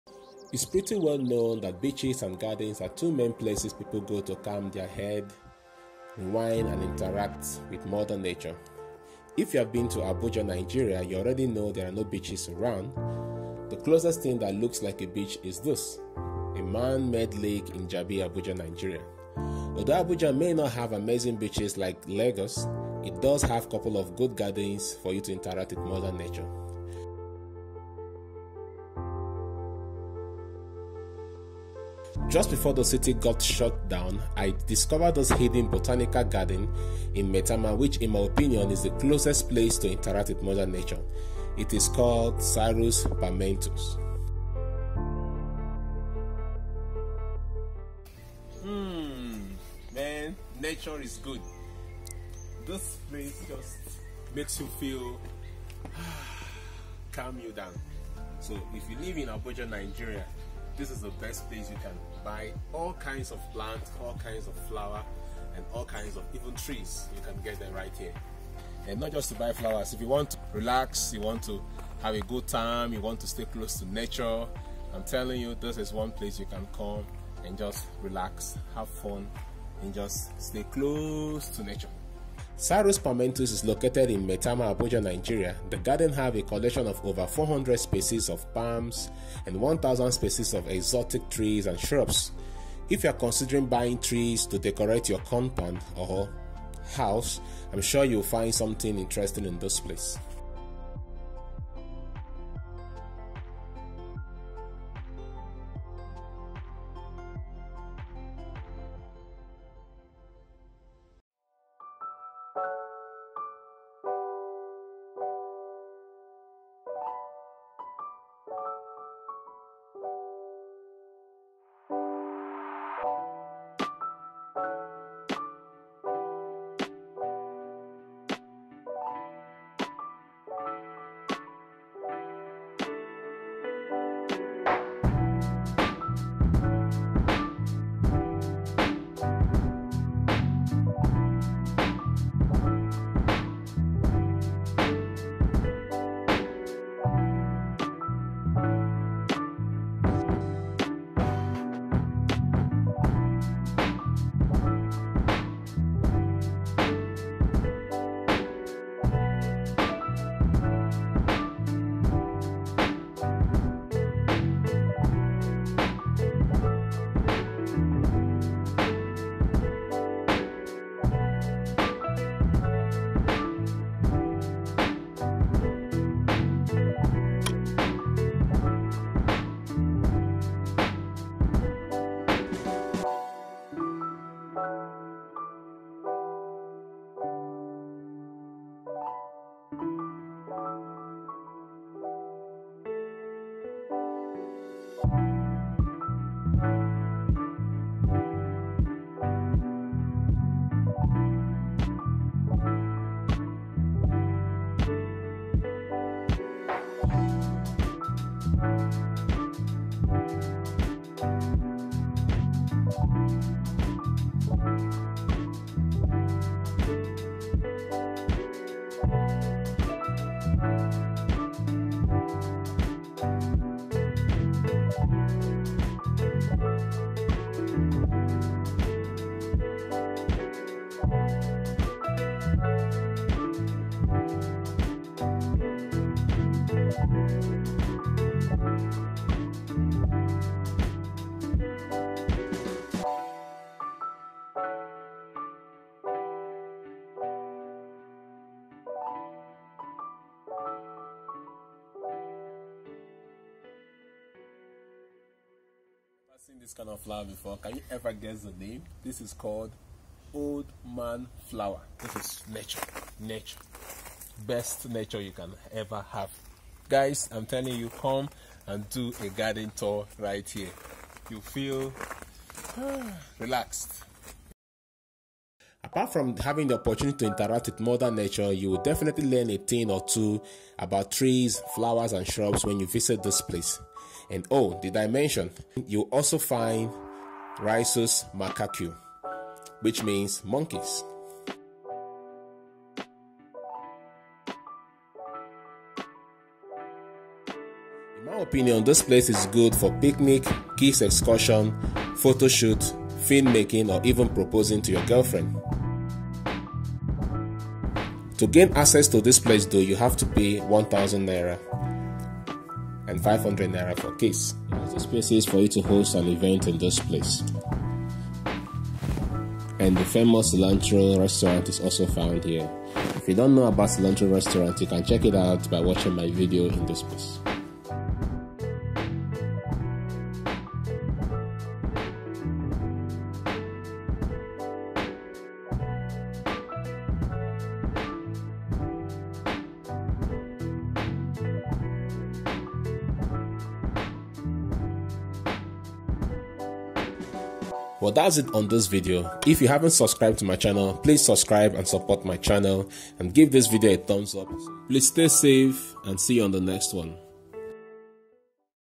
The Biggest Botanical Garden in Abuja, Nigeria
I shot this video and all my other videos on Xiaomi Redmi Note 7

This video is about the biggest botanical garden in Abuja, Nigeria. Located in Maitama, before the cilantro restaurant. It is called Sarius Palmetum

 Sarius Palmetum has over 1000 different species of trees, palms, and flowers. The garden is ideal for recreational activities as well, Hope you like it